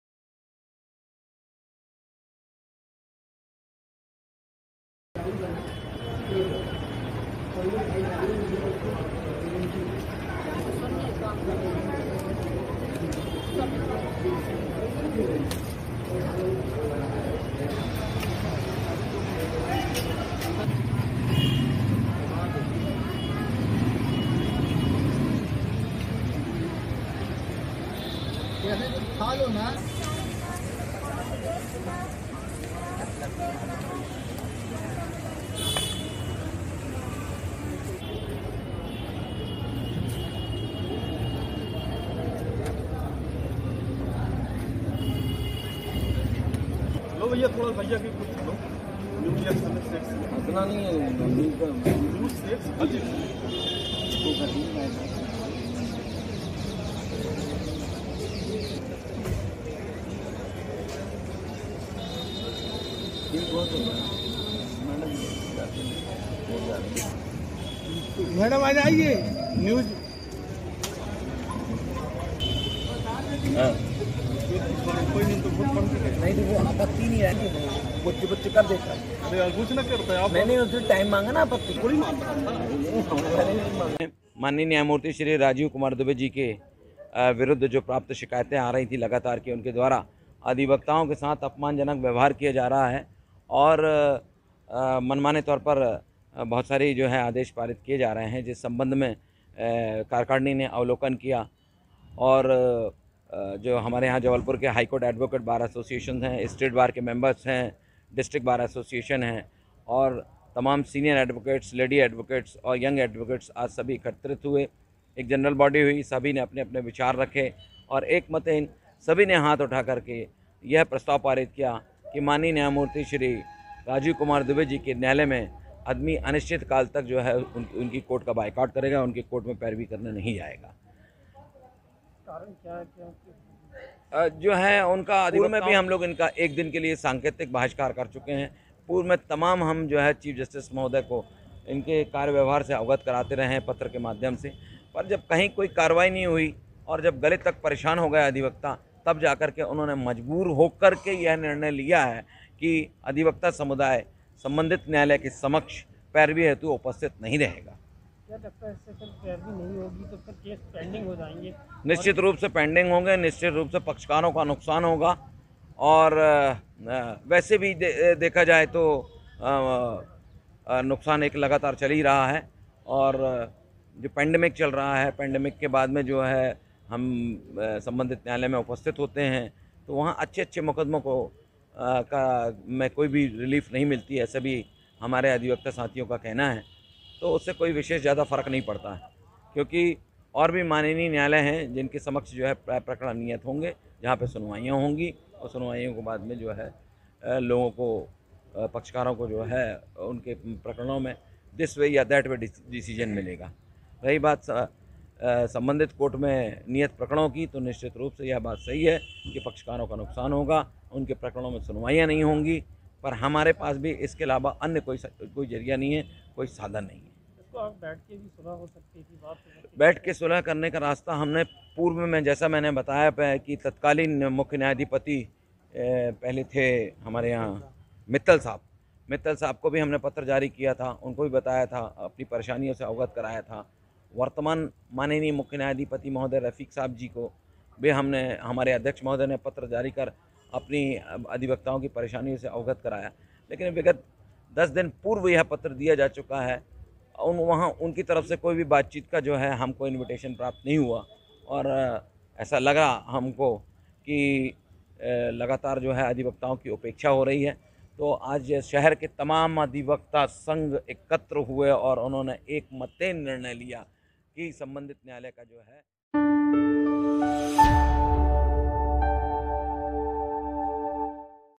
MP High Court Bar Association के Lawyers ने Justice Rajiv Kumar Dubey की अदालत का किया बहिष्कार
एमपी हाईकोर्ट बार एसोसिएशन ने जस्टिस राजीव  कुमार दुबे के न्यायालय का अनिश्चितकाल के लिए बहिष्कार कर दिया है। बार एसोसिएशन के लोगों का कहना है कि न्यायमूर्ति राजीव  कुमार दुबे के खिलाफ अधिवक्ताओं की तरफ से लगातार शिकायतें की जा रही हैं।